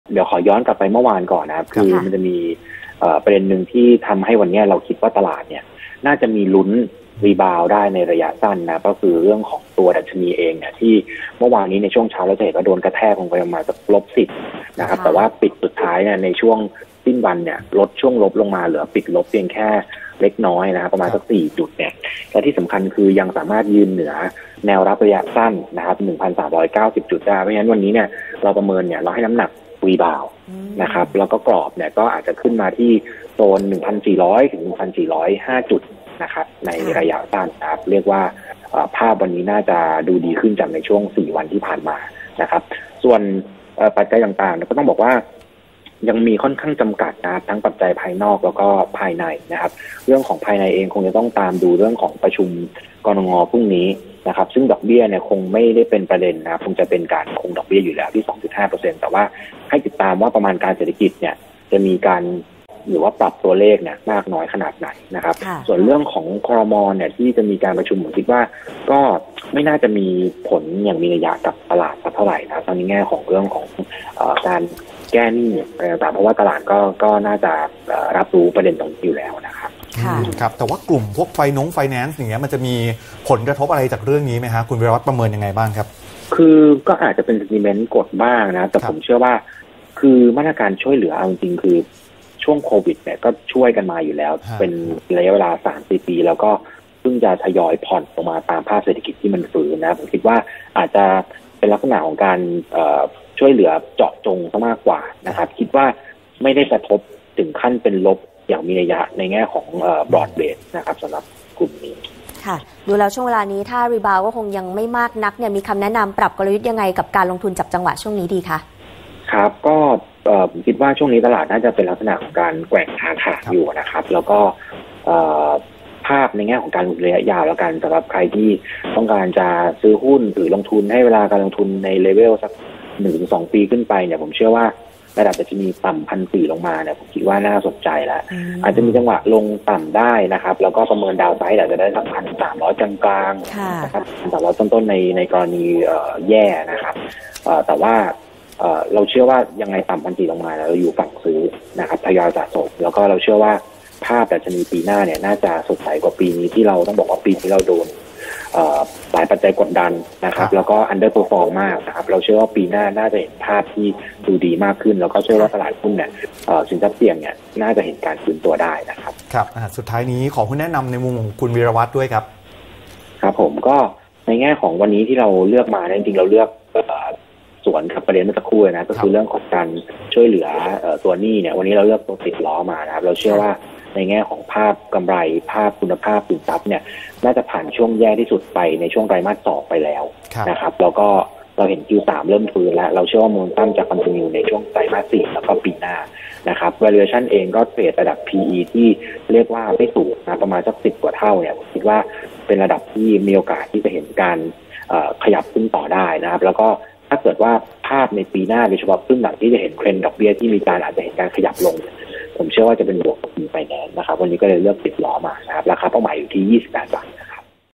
ปัจจัยหนุนยังมีจำกัด | นาทีลงทุน 28 พ.ย.66
ปัจจัยหนุนยังมีจำกัด แต่ตลาดยังรีบาวด์ได้ แนวต้าน 1,400-1,405 จุด นักลงทุนจับตาการประชุม กนง. ซึ่งคาดยังคงดอกเบี้ยต่อไป